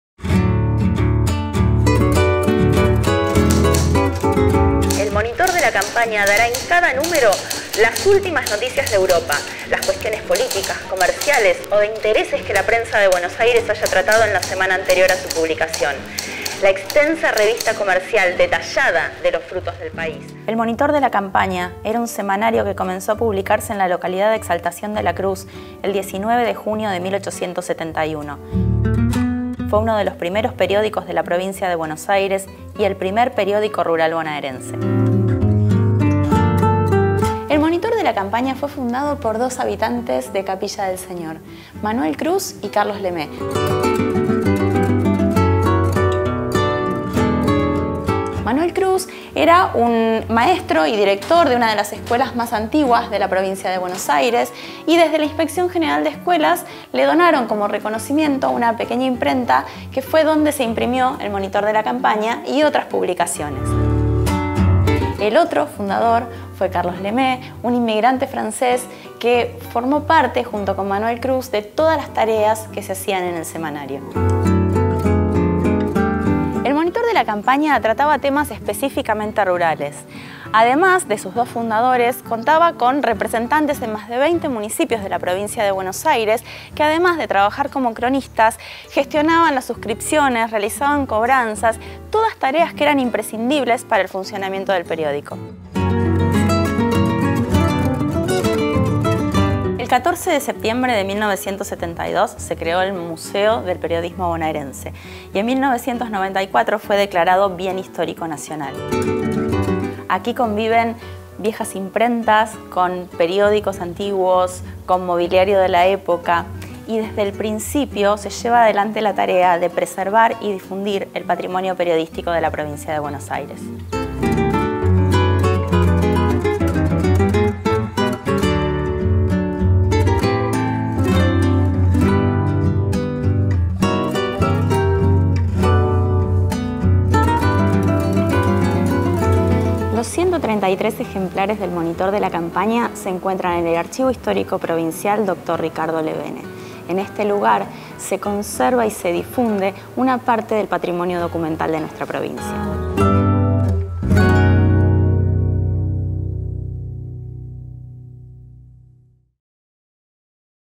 EL ORIGEN DEL PERIODISMO BONAERENSE
La Profe Eugenia Younis nos trae la historia del Museo del Periodismo Bonaerense, nos muestra uno de los primeros periódicos provinciales y los hojea en el Archivo Histórico Provincial.